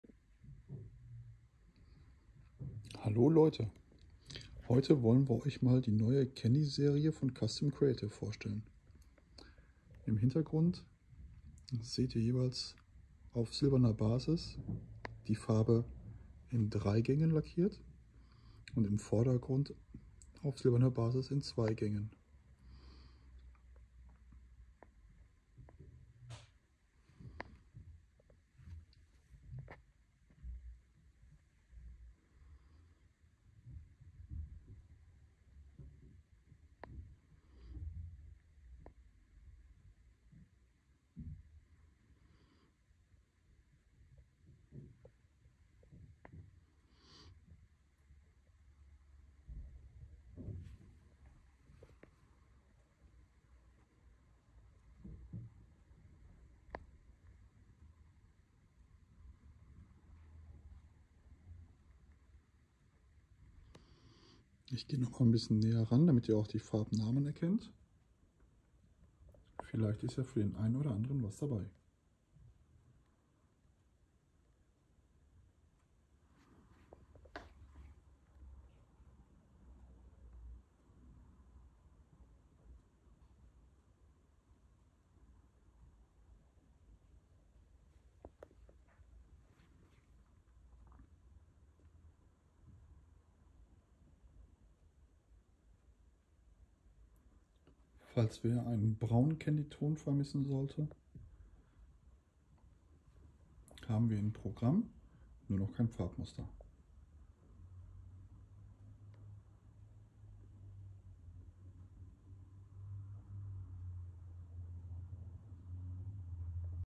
Custom Creative Kandykonzentrate auf silber lackiert
Hier seht ihr die neuen Kandykonzentrate von Custom Creative. Auf den Mustern wurden sie auf silbernen Basis lackiert.
Auf den vorderen Mustern sind zwei Gänge lackiert worden und auf den hinteren drei Gänge Hier findet ihr den Link zu den Kandy Konzentraten.